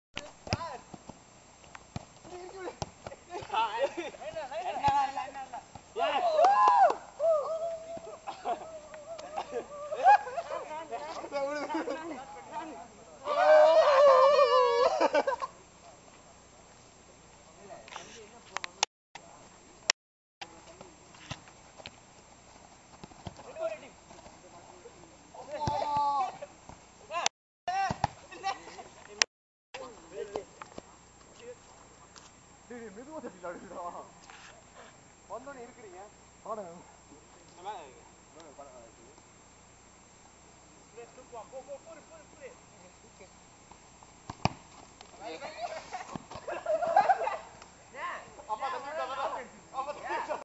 indian futsal
unimas indian students playing futsal at gunung gading